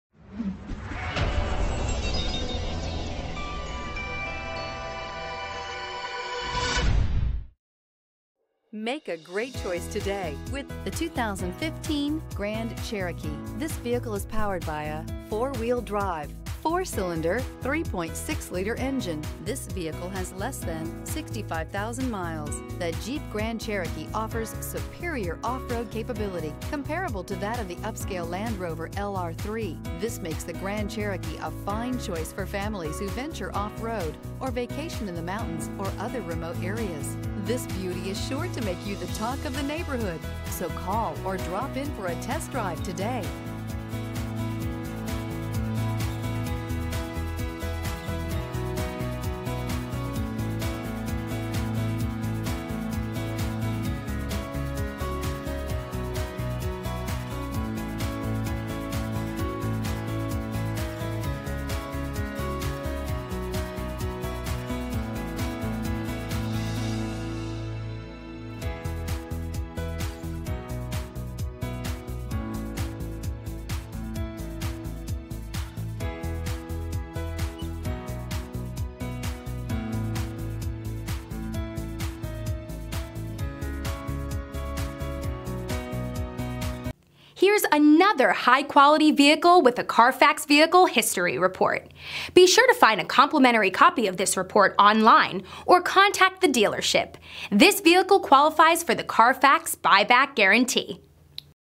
2015 Jeep Grand Cherokee 91121a
2015 Jeep Grand Cherokee Limited

Flagship Ford
850 Fern Dr.

Yes, only 61,855 miles on this 2015!  Looking for a FULLY loaded Limited?  This one has it all, and in the best color, Brilliant Black Crystal Pearl Coat with matching black heated and cooled leather seats.  Local trade, clean CarFax.  Went through full inspection with flying colors and is ready for its new home.